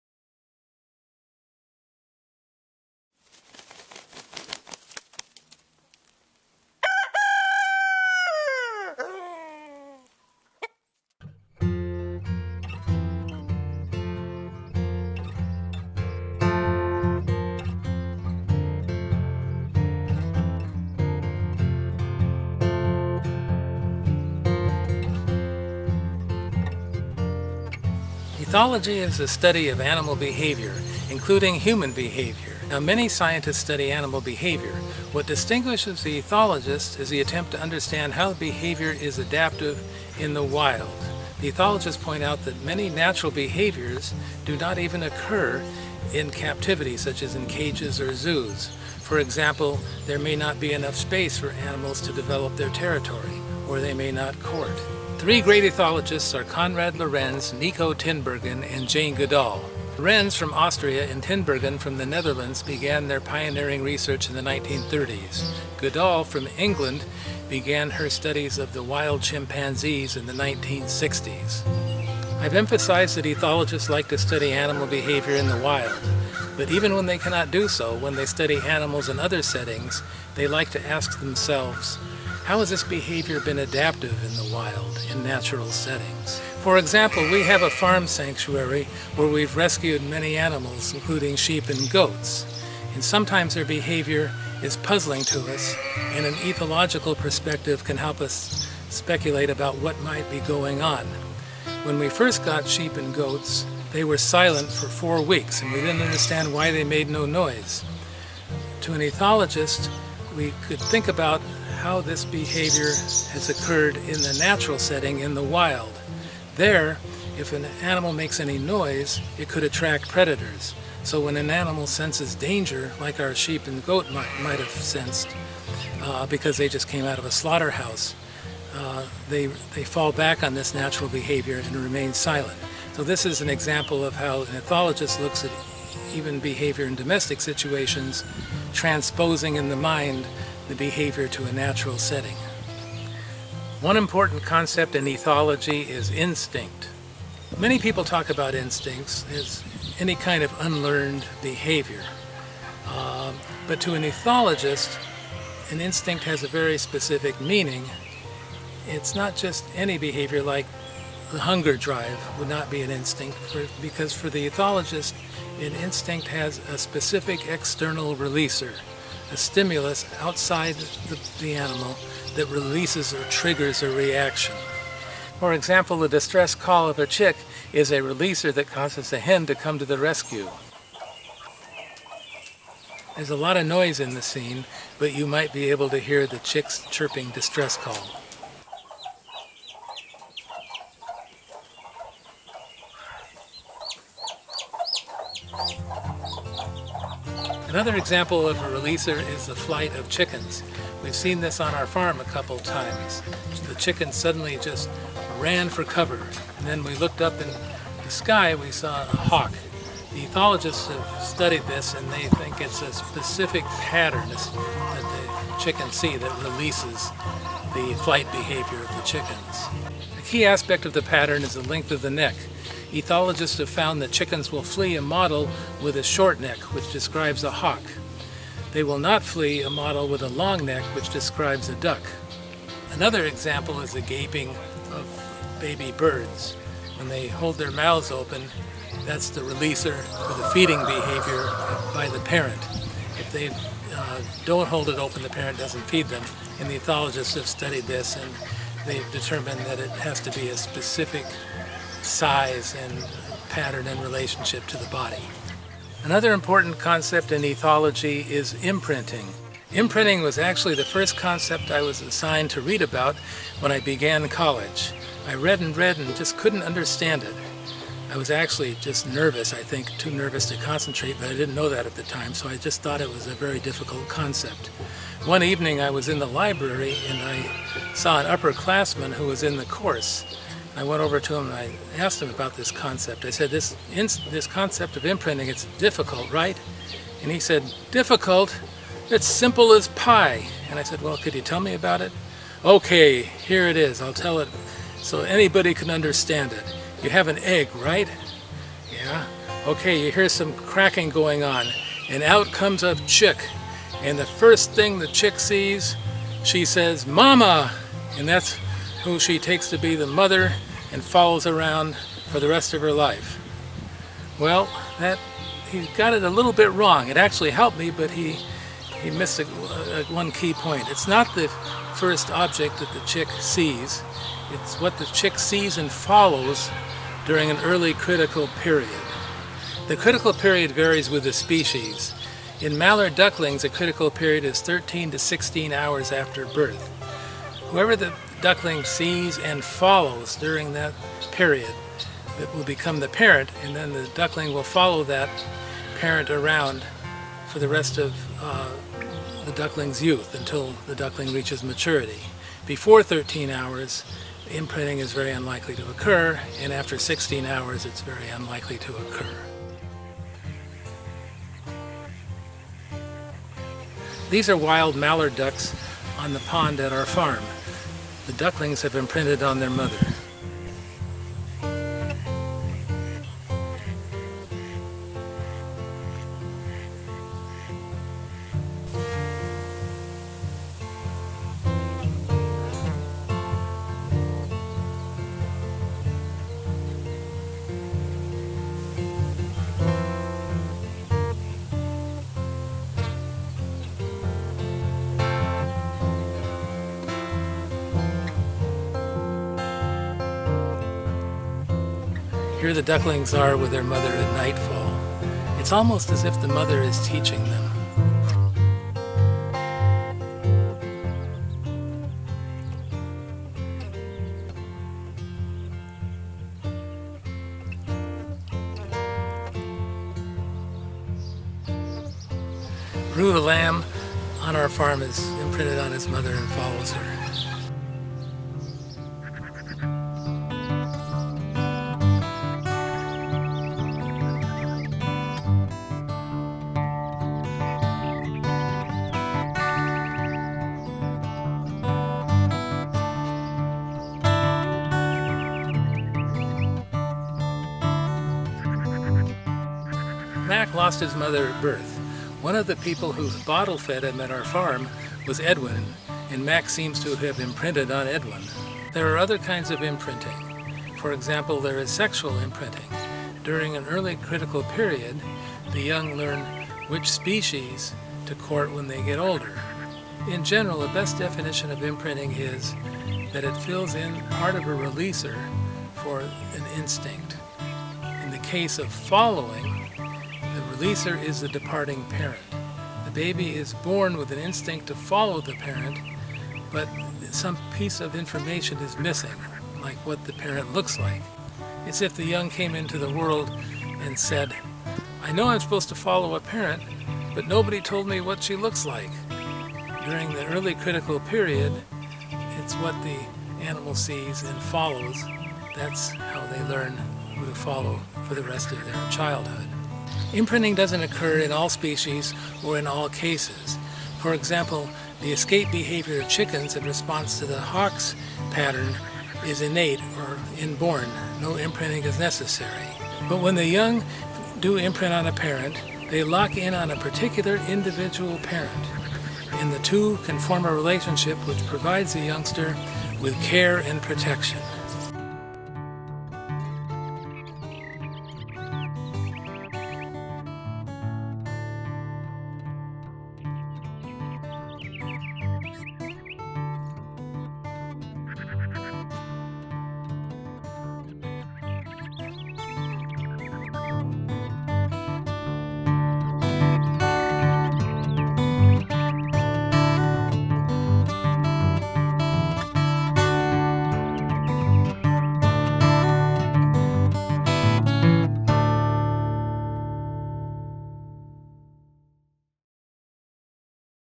Ethology: Instinct & Imprinting
A professor's lecture includes examples of animal behavior at his farm sanctuary.